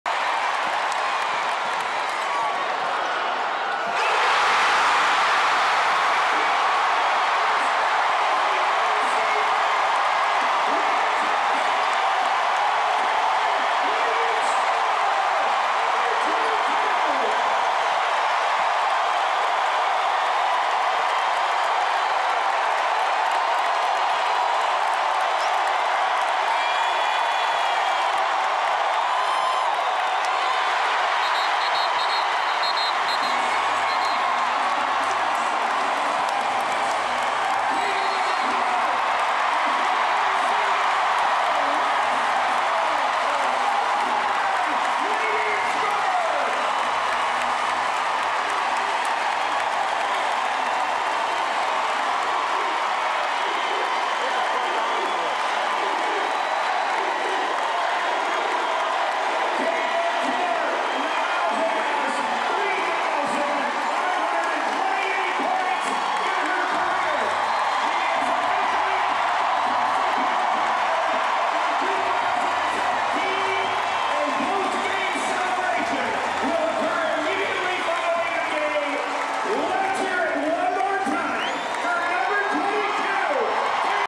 WATCH: Caitlin Clark's record-breaking shot vs. Michigan
Caitlin Clark made a 3-pointer to pass Kelsey Plum as the NCAA's all-time women's basketball scorer.